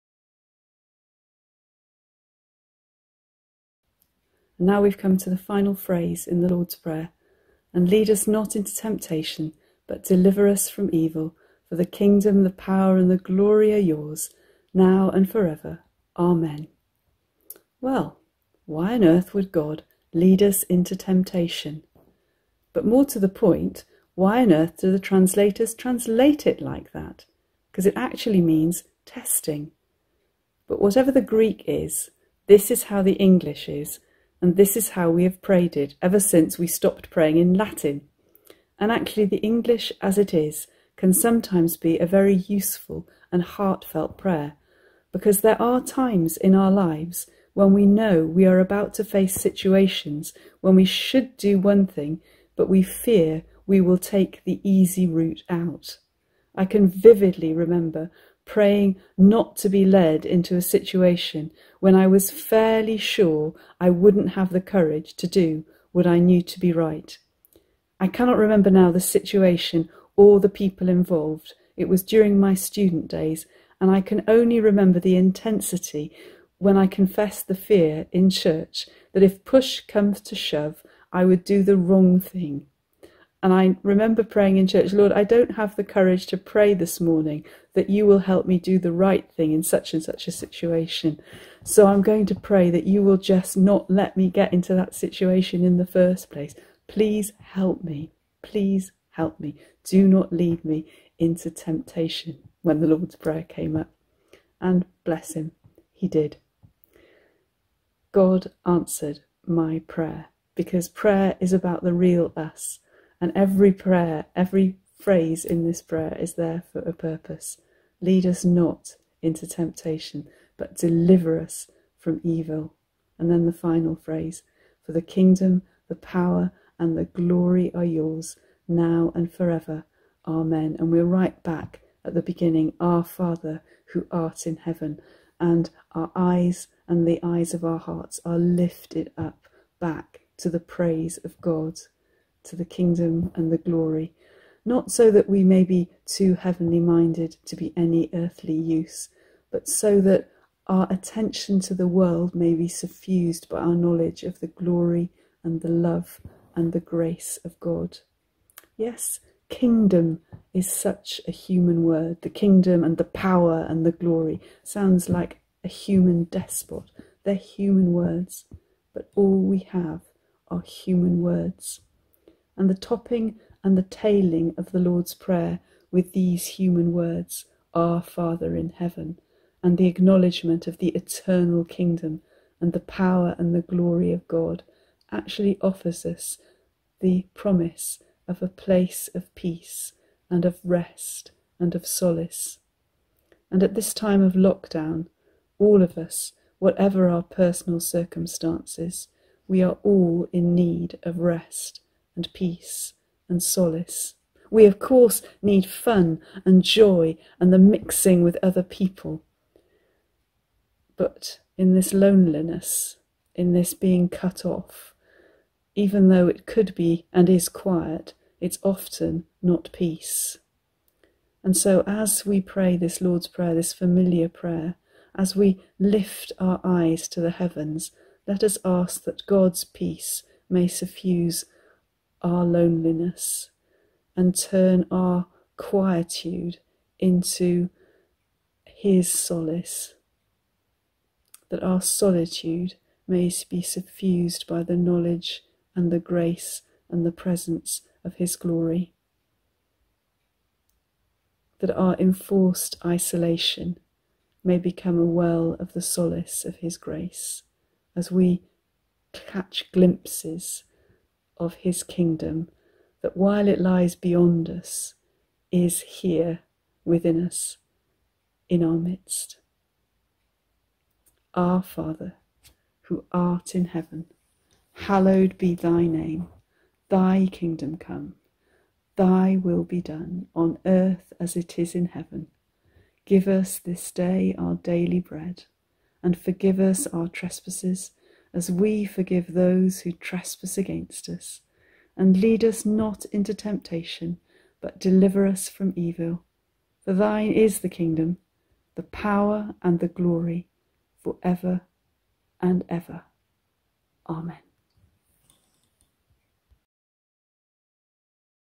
My Prayer Journey
Wednesday February 24th
Daily reflections for Lent with the Bishop of St Davids, Rt Revd Dr Joanna Penberthy
The Lord's Prayer: Lead us not into temptation...